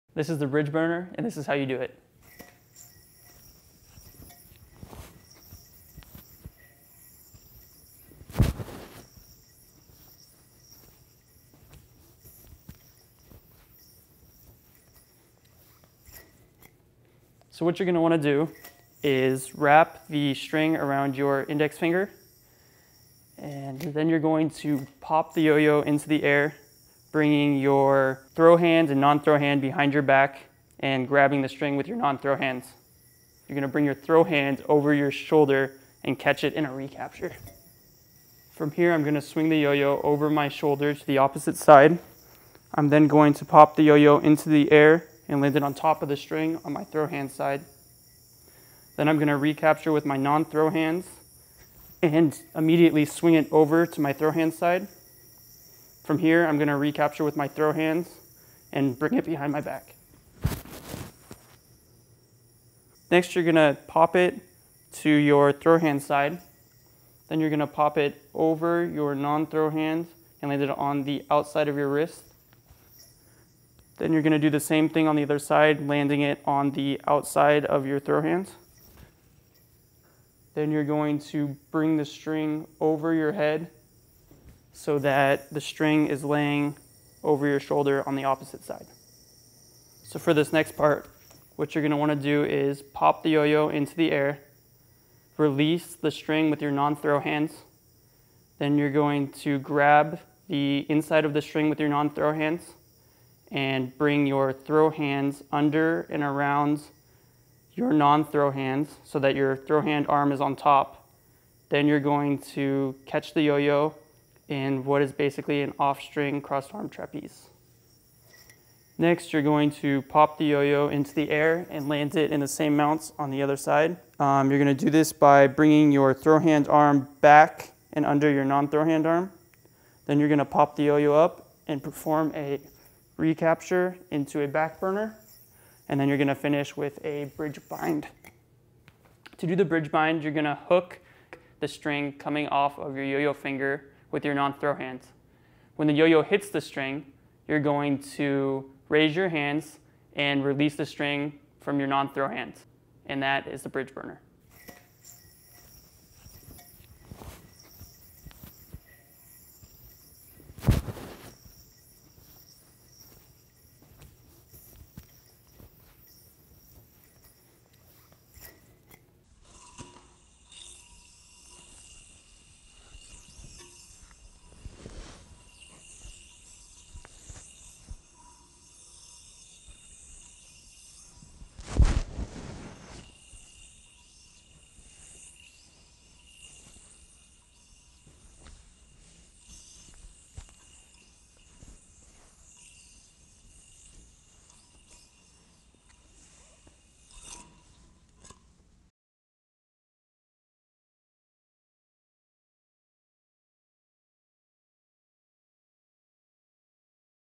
The Bridge Burner Offstring Trick - Zac Rubino Tutorials
Learn to do the Bridge Burner Offstring Yoyo Trick from Zac Rubino
Bridge Burner is an advanced 4A trick which incorporates a lot of shoulder action and front to back arm transfers.